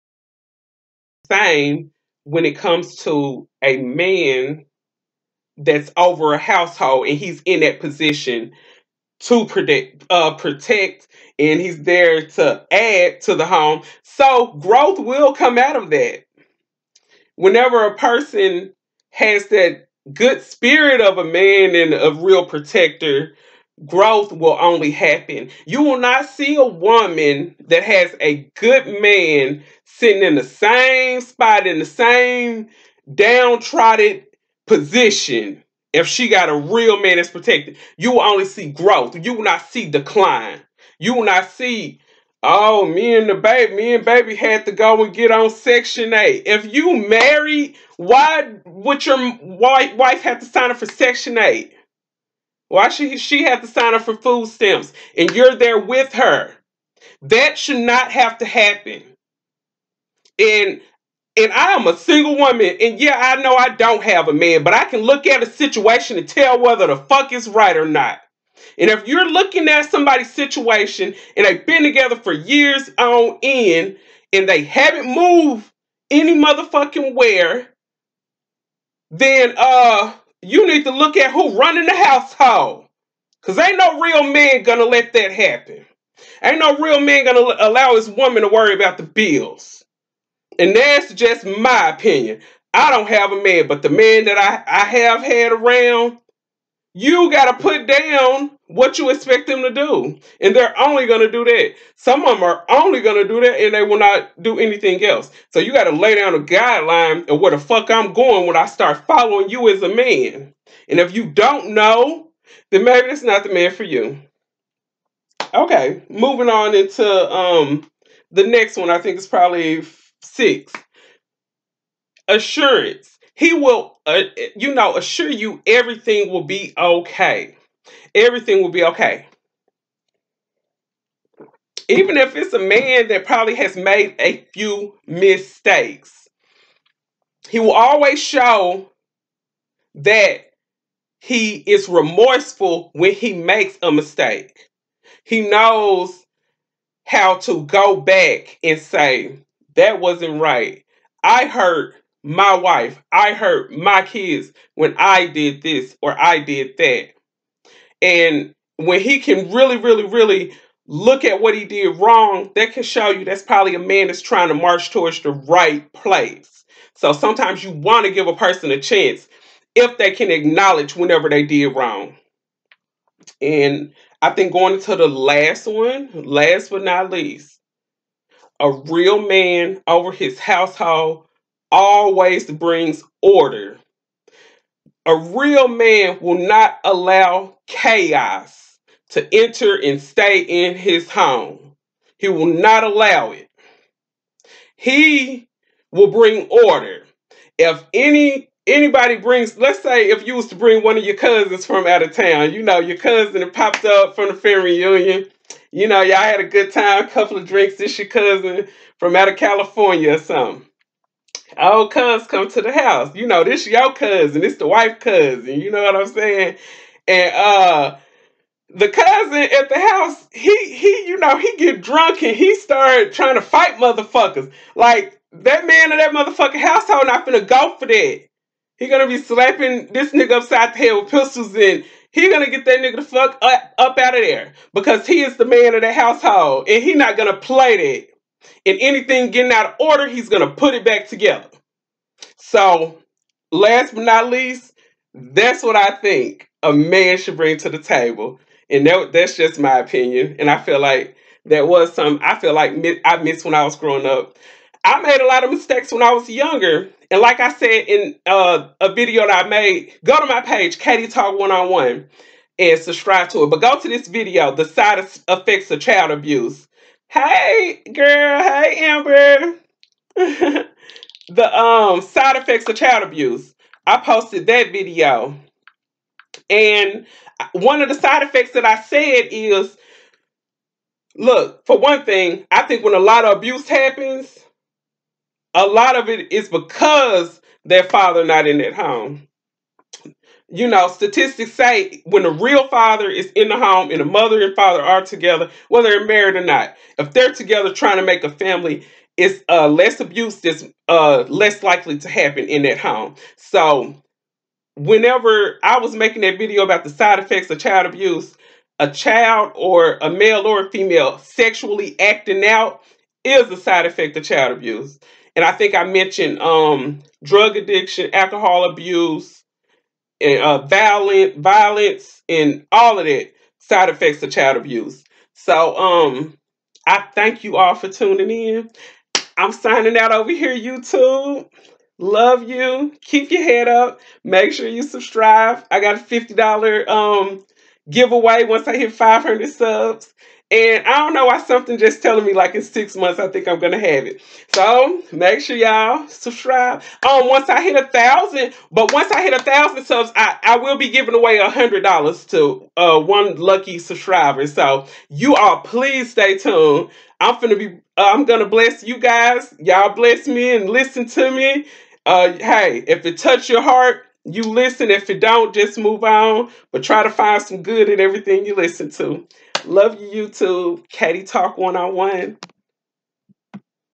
The importance of the man's role in the household!!!
Top six things that good fathers bring to the household.